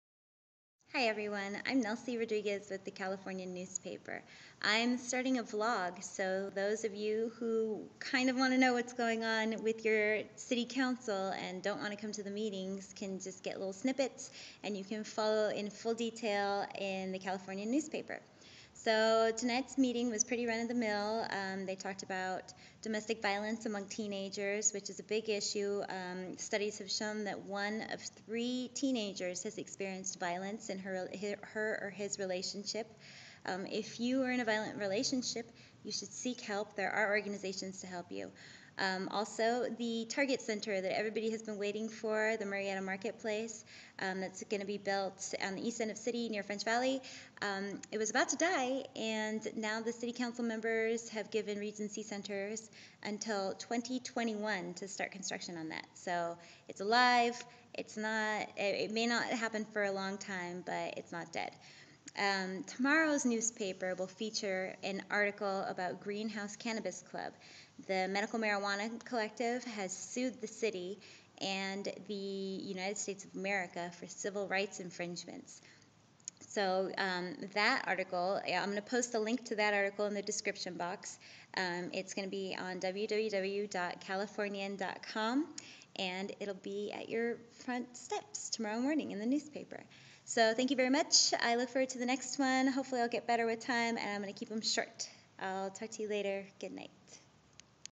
I'll cover Murrieta, you do you.
Raw commentary of Feb. 21, 2012 Murrieta CA city government meeting.
"Medical marijuanal collective director files civil rights suit against city"